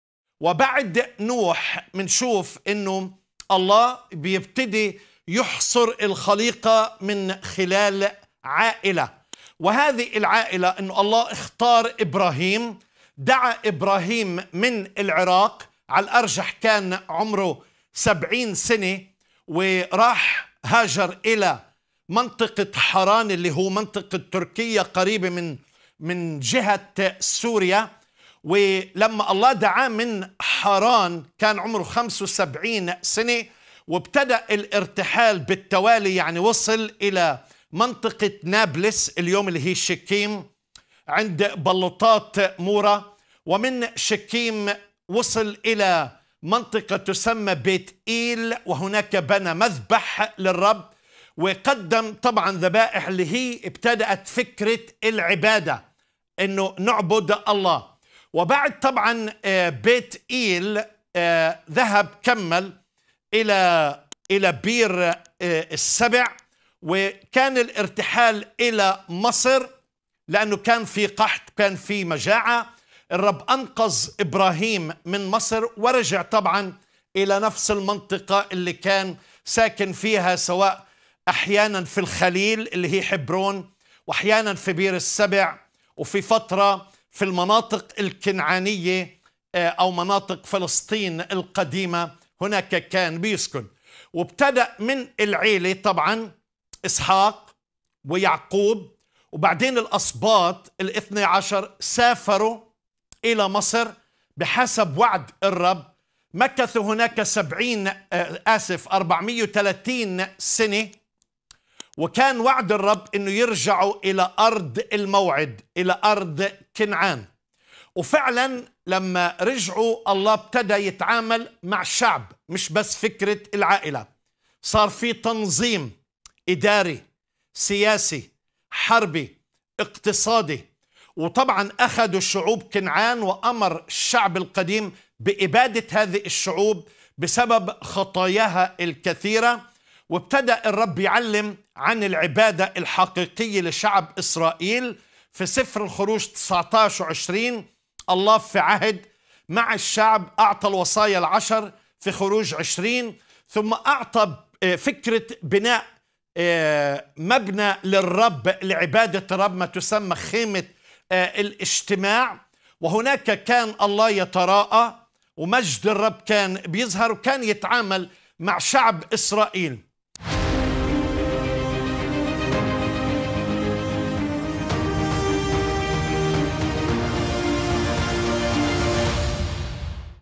دعوة الله لإبراهيم - من العائلة إلى الأمة، قصة شعب الله
📌 دعوة الله لإبراهيم
🌟 اختيار إبراهيم: الله يختار إبراهيم ليكون أباً لأمة عظيمة.
🚶‍♂️ الرحلة والوعد: رحلة إبراهيم وأسرته من العراق إلى كنعان، ووعد الله بإعطائهم هذه الأرض.
🏞️ التكاثر والاستقرار: تكاثر نسل إبراهيم وإسحاق ويعقوب، واستقرارهم في كنعان ثم انتقالهم إلى مصر.
🌊 الخروج من مصر: خروج بني إسرائيل من مصر بقيادة موسى وتلقيهم الشريعة على جبل سيناء.
🌈 العهد الجديد: الله يعقد عهداً جديداً مع شعبه، ويختارهم ليكونوا شعباً خاصاً له.

📌يمكنك التواصل معنا بصورة شخصية عن طريق رسائل الواتساب👇
WhatsApp الواتساب:    🆆🅷🅰🆃🆂🅰🅿🅿